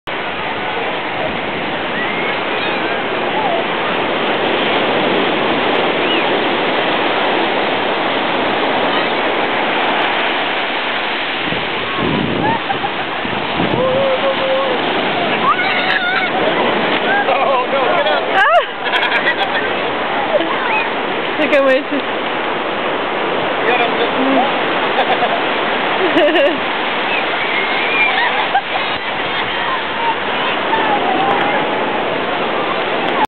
Coronado Tsunami
The Playmans enjoying the tide at Coronado Beach.